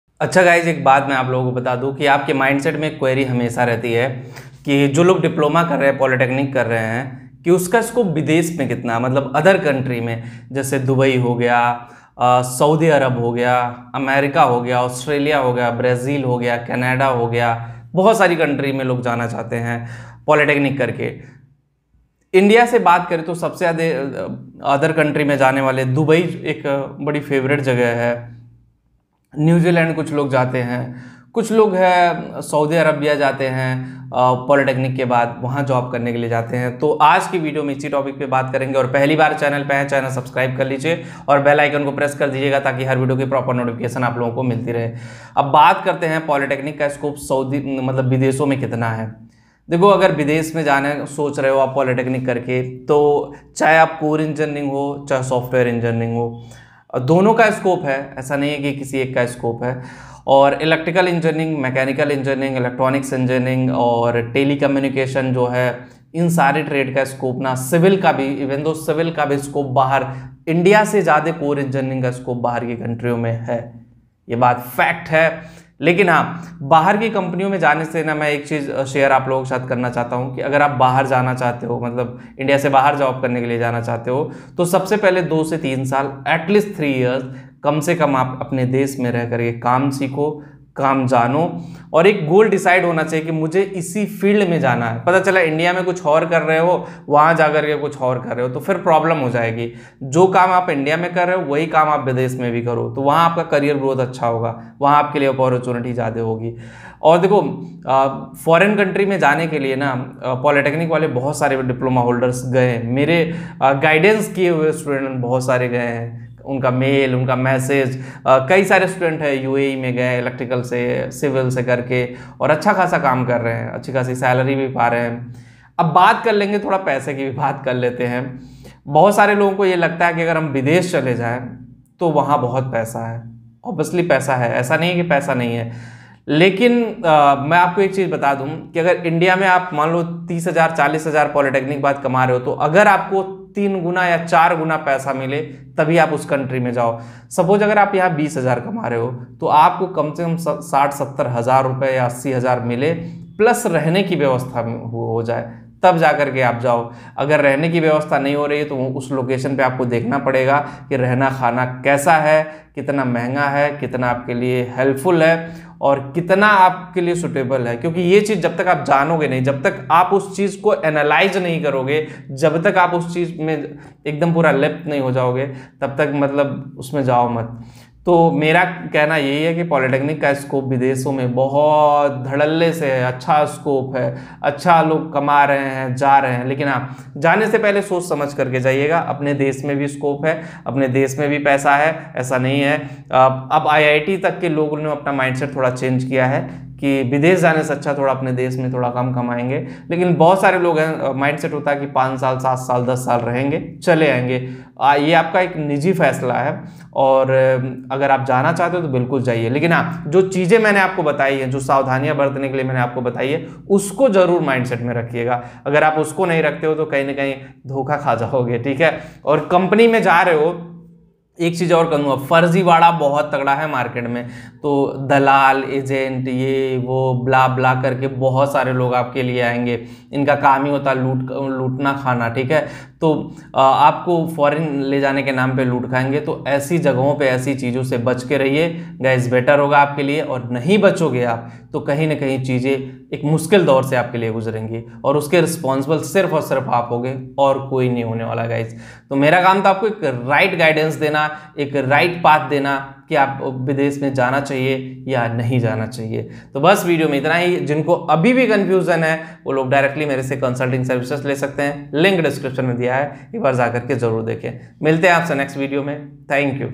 पॉलिटेक्निक का स्कोप विदेश में कितना है | Diploma Scope in Foreign Countries 🔥🔥😯
Diploma scope in Foreign Countries me kitna hai is video me maine aap sab logo ko yahe bataya hai.